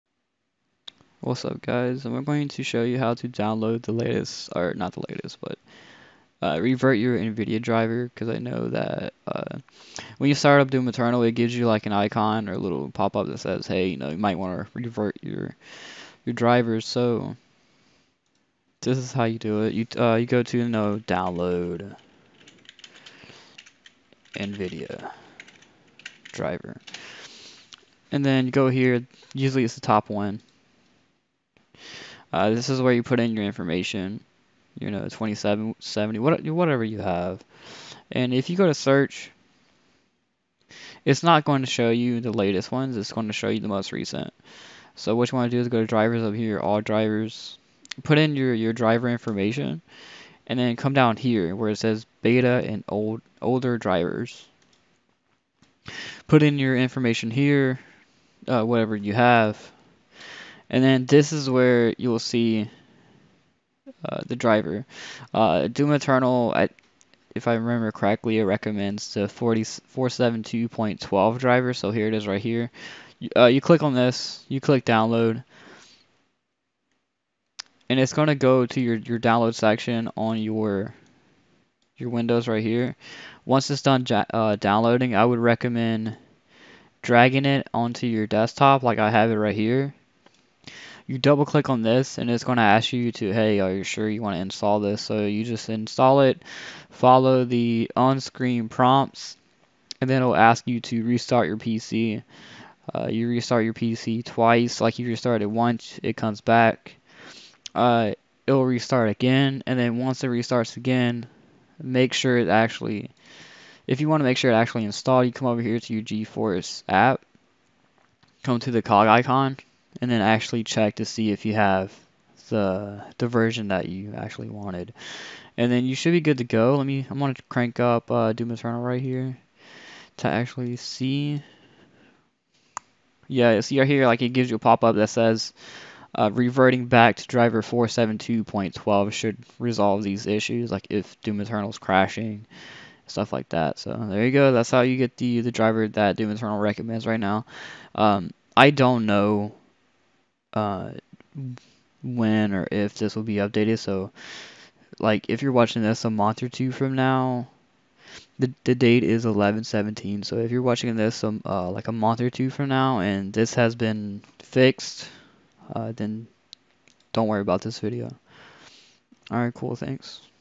How to revert your graphic driver to 472.12 for Doom Eternal
Just a lil video on how to revert your graphic driver back to 472.12 for Doom Eternal if you are experiencing crashes and other bugs